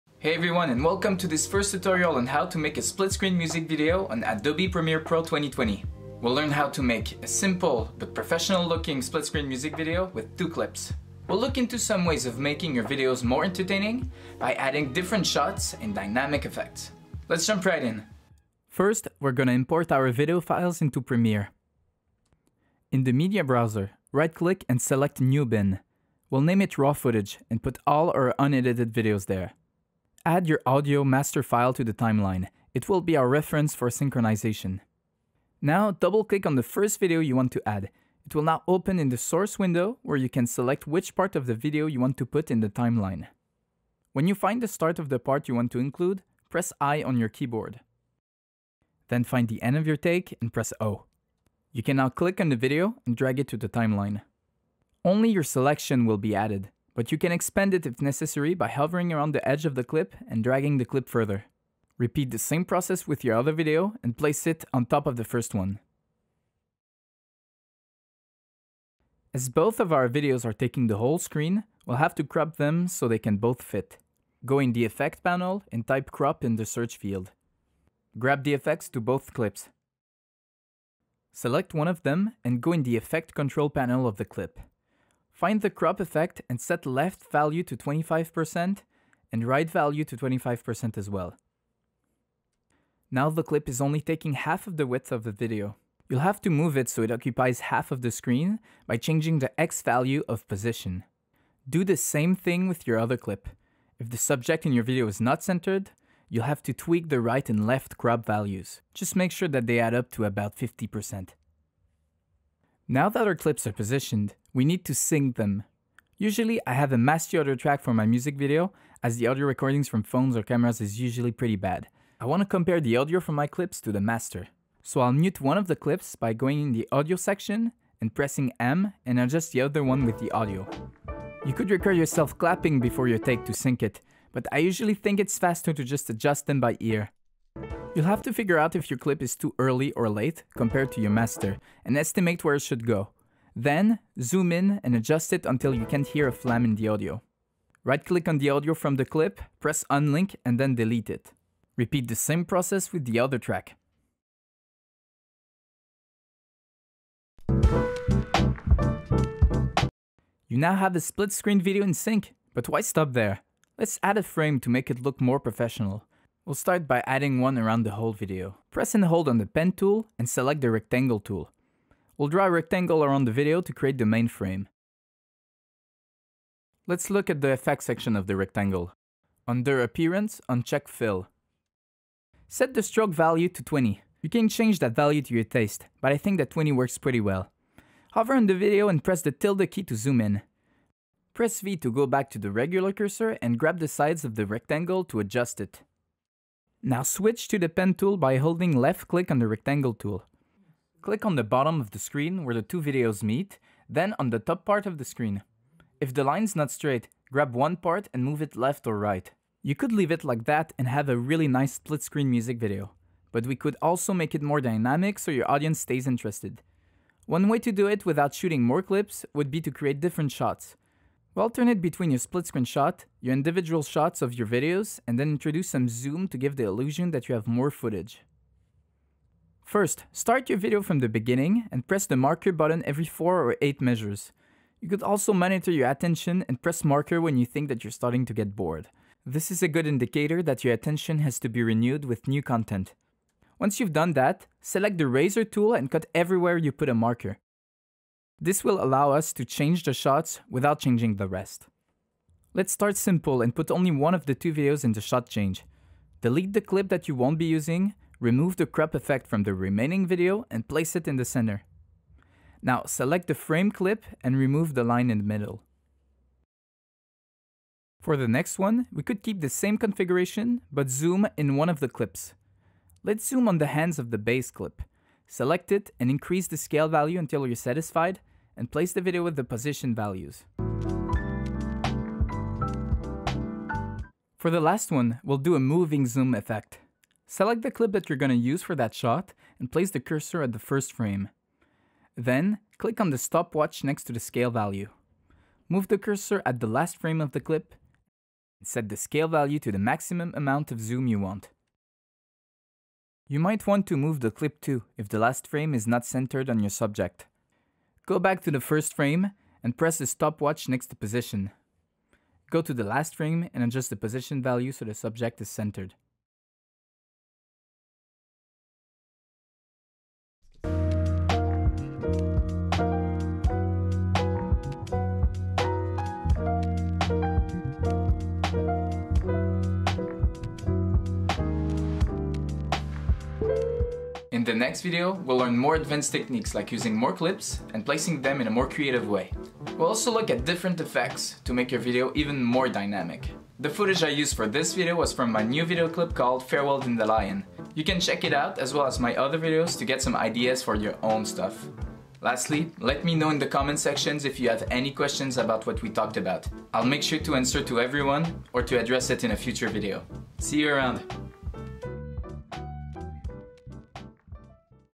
HOW TO make a SPLIT SCREEN music video \\ Adobe Premiere Pro 2020 tutorial
Here's the first part of a series of tutorials on how to make split-screen music videos on Adobe Premiere Pro 2020. We discuss how to make a basic video with two clips side by side, how to add a frame around the clips and how to add some effects and shots to make it more dynamic and improve viewer retention.

I started making music videos around the beginning of quarantine. I had to learn so much in order to get there, a lot on my own because there aren't many tutorials on video editing for musicians. That's why I thought it would be great to share it with you. As a piano teacher, it made a lot of sens to start these types of videos.

Make sure to leave your questions about video editing and anything related to music videos in the comment section. I'll try to answer to every comment.